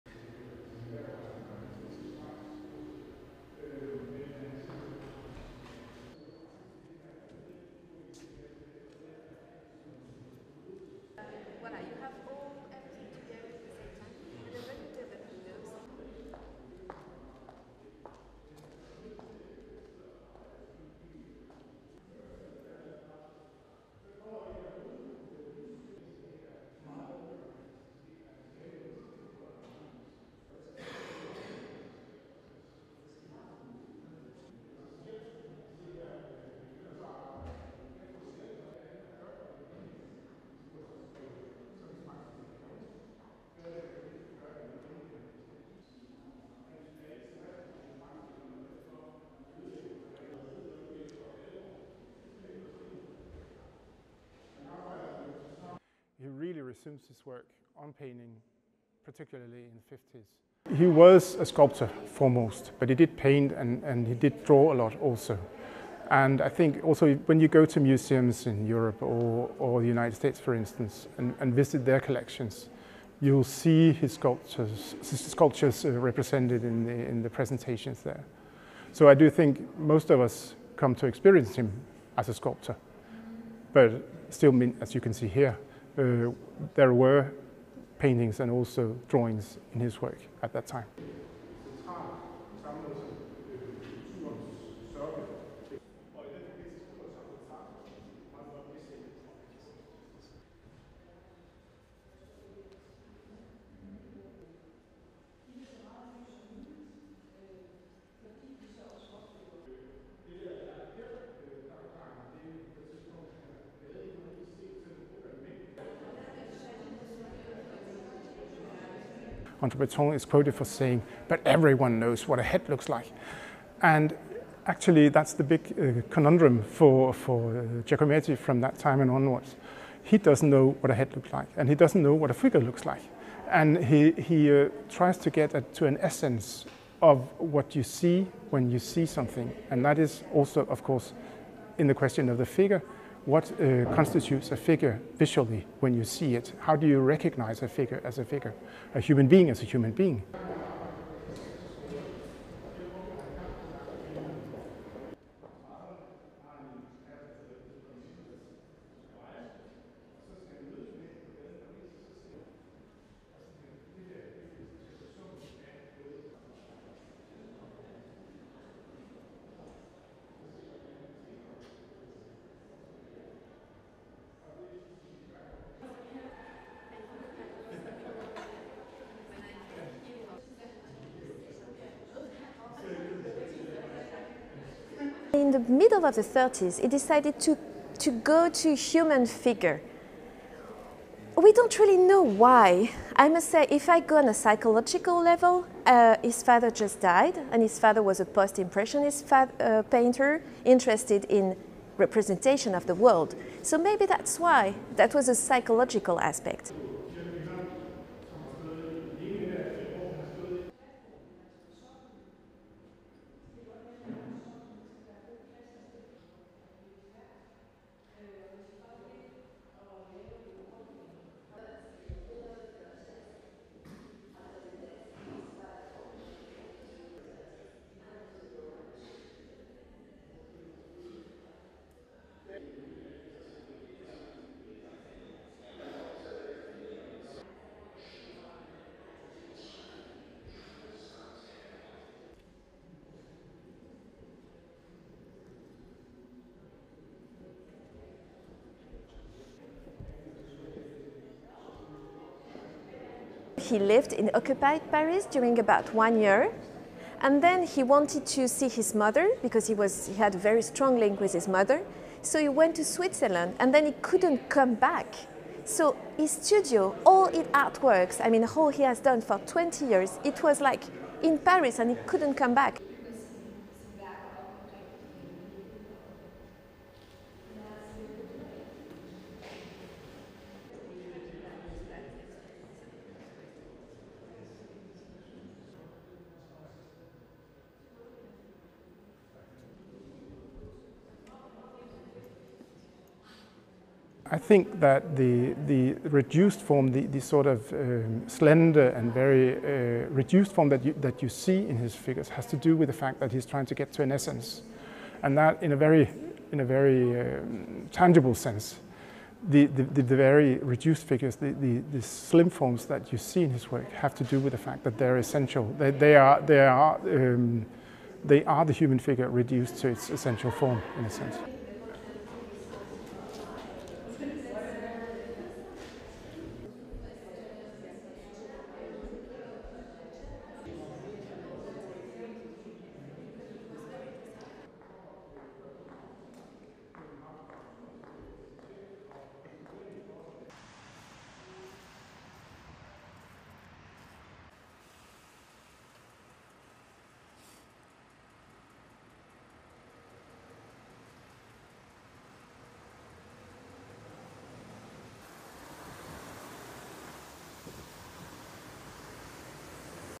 What Meets the Eye: Copenhagen exhibit explores Giacometti’s quest for physical reality
DENMARK GIACOMETTI EXHIBIT

RESTRICTION SUMMARY:

LENGTH: 6:00

ASSOCIATED PRESS
Copenhagen, Denmark - 7 February 2024

1. Pan right of “The Nose” 1947 on display at SMK’s new exhibit “Alberto Giacometti - What Meets the Eye”
2. Various of “Tall Thin Head” 1954
3. Pan right of “Walking Man II” 1960
4. “Falling Man” 1950
5. Various of “The Cage” 1950-51
6. Tilt up of “Three Walking Men” 1948
7. Various of “Four Figurines on a Stand” 1950
8. Setup shot of chief curator Thomas Lederballe Pedersen
9. SOUNDBITE (English) Thomas Lederballe Pedersen, chief curator:
"He was a sculptor foremost, but he did paint, and he did draw a lot also. And I think also, when you go to museums in Europe or the United States, for instance, and visit their collections, you'll see his sculptures represented in the presentations there. So, I do think most of us come to experience him as a sculptor, but still, as you can see here, there were paintings, and also drawings in his work at that time."

10. Pan left of “Tall Thin Head” 1954
11. Pan right of artworks on display
12. Various of “Bust of a Seated Man” 1965
13. Tilt down from “Bust of Diego” 1964 to “Bust of a Man (Known as ‘New York II’)” 1965
14. Mid of several artworks
15. SOUNDBITE (English) Thomas Lederballe Pedersen, chief curator:
"André Breton (French writer and poet) is quoted for saying; 'But everyone knows what a head looks like.' And actually, that's the big conundrum for Giacometti from that time and onwards. He doesn't know what a head looks like, and he doesn't know what a figure looks like, and he tries to get to an essence of what you see when you see something. And that is also, of course, in the question of the figure, what constitutes a figure visually when you see it. How do you recognize a figure as a figure, a human being, as a human being?"

16. Pan right of artworks on display
17. Tilt up of “Head of the Father” 1927-30
18. “Head of the Mother” 1927
19. Various of “Bust of a Man” 1962
20. Various of “Walking Limping Figure” 1930
21. SOUNDBITE (English) Emilie Bouvard, director of collections and scientific programmes, Fondation Giacometti:
"The middle of the (19)30s it decided to go to human figure. We don't really know why. I must say, if I go on a psychological level, his father just died, and his father was a post-impressionist painter interested in representation of the world. So, maybe that's why that was a psychological aspect."

22. Various of “Black Annette” 1962
23. Various of sketches on the review Les Temps Modernes, June 1954
24. Various of “Very Small Figurine” 1937-39
25. SOUNDBITE (English) Emilie Bouvard, director of collections and scientific programmes, Fondation Giacometti:
"He lived in occupied Paris during about one year, and then he wanted to see his mother because he had very strong link with his mother. So, he went to Switzerland and then he couldn't come back. So, his studio, all his artworks. I mean, the whole he has done for 20 years, it was in Paris, and he couldn't come back."

26. Pull focus from “Small Head of Diego on Double Base’ 1936-37 to “Head of Woman (Rita)” 1936
27. “Figurine in a Box Between Two Boxes That Are Houses” 1950
28. “Head of Man face” 1956-57
29. “Tall Thin Head” 1954, visitor walking by
30. SOUNDBITE (English) Thomas Lederballe Pedersen, chief curator:

31. Pan right of exhibit
32. Artworks, “Bust of a Seated Man” 1965 in background
33. Artworks, visitor in background
34. Pan left from “Walking Man II” 1960 to “Three Walking Men” 1948
36. Pan left of Statens Museum for Kunst exterior